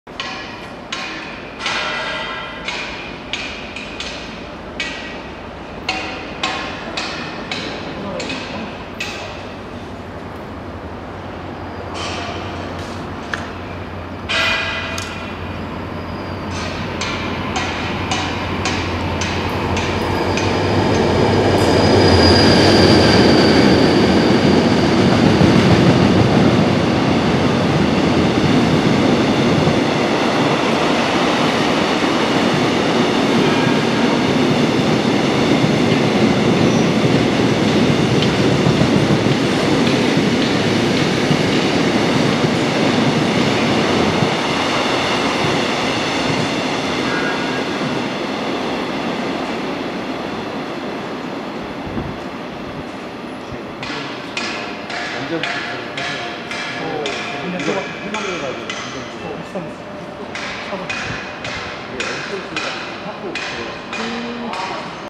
장항선 아산역을 통과하는 부산신항행 컨테이너 화물열차 #3045
[4K[ 장항선 아산역을 통과하는 부산신항행 컨테이너 화물열차 #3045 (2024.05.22)
DL7455견인